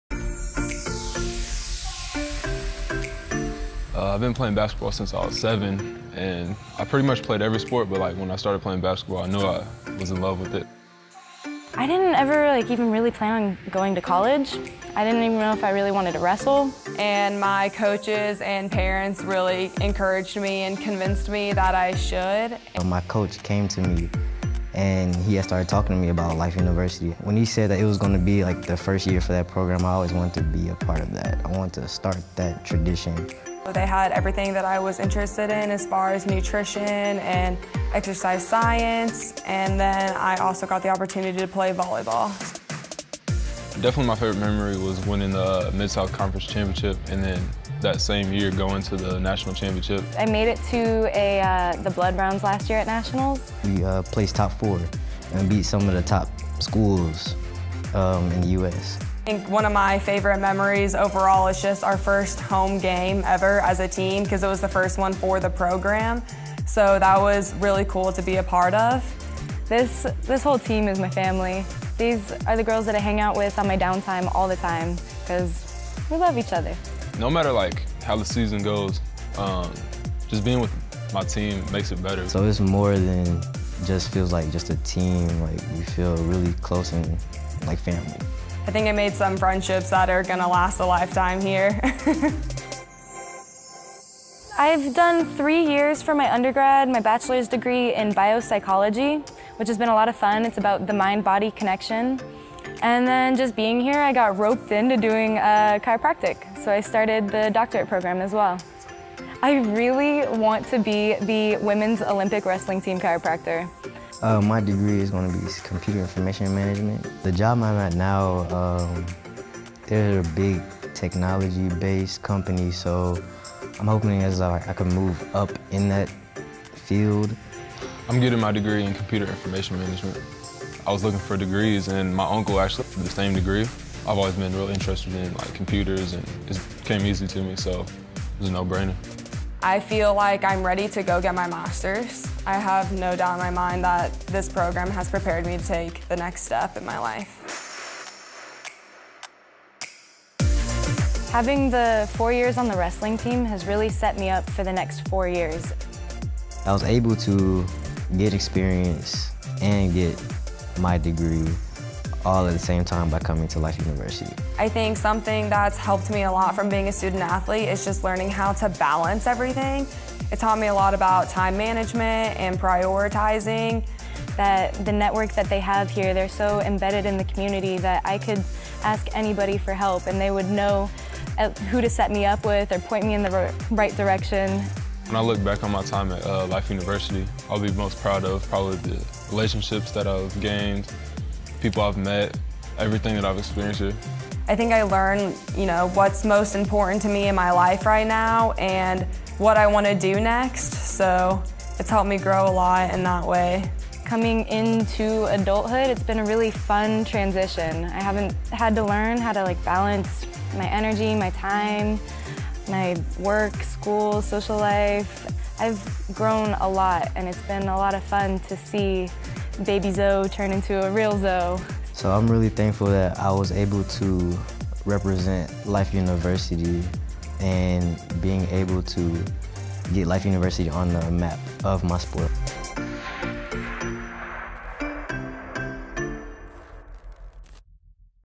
LIFE U Athletics Seniors 2019
We sat down with three seniors in the department to share their experiences and why they chose Life University!